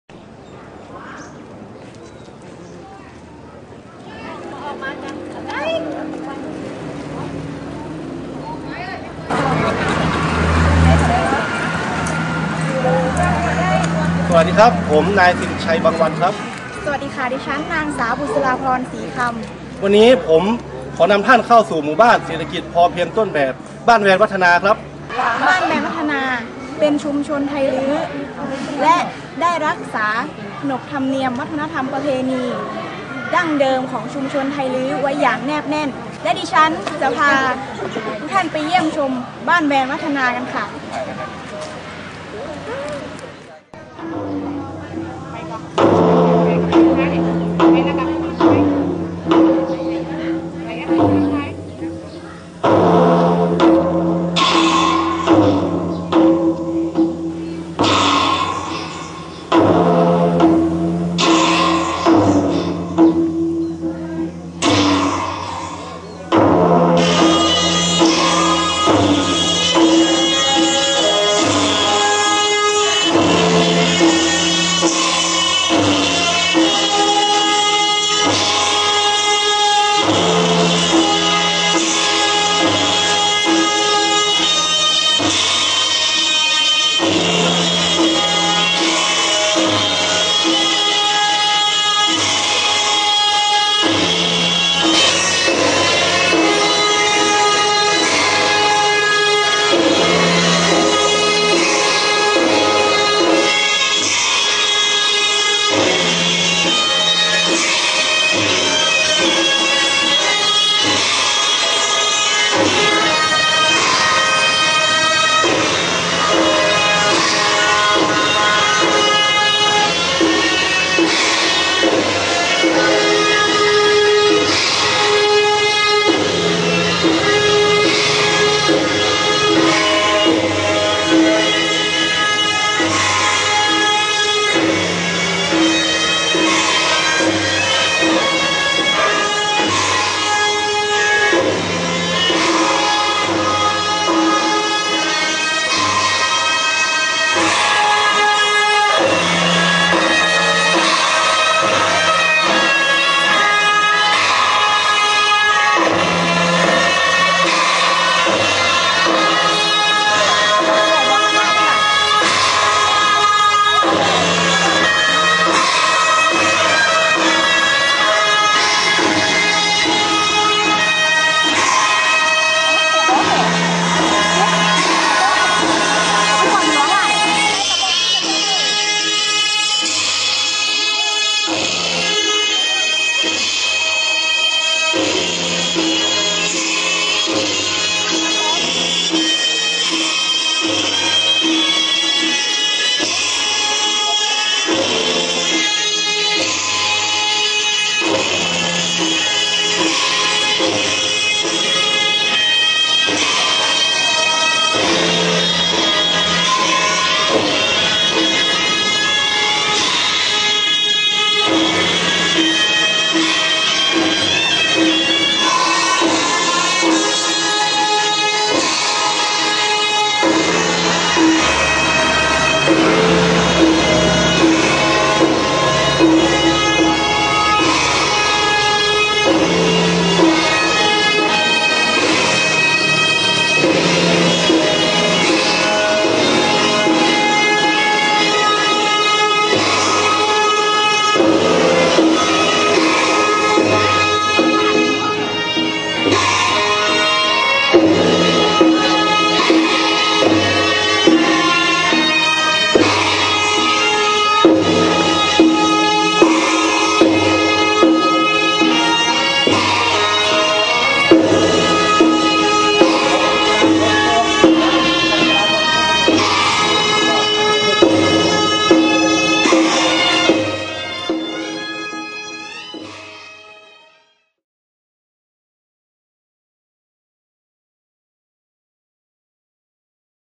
ฟ้อนชาวไทลื้อบ้านแวนวัฒนา
วัฒนธรรมการชาวไทลื้อ "ฟ้อนชาวไทลื้อบ้านแวนวัฒนา" บ.แวนวัฒนา ม.๕ ต.เชียงบาน อ.เชียงคำ จ.พะเยา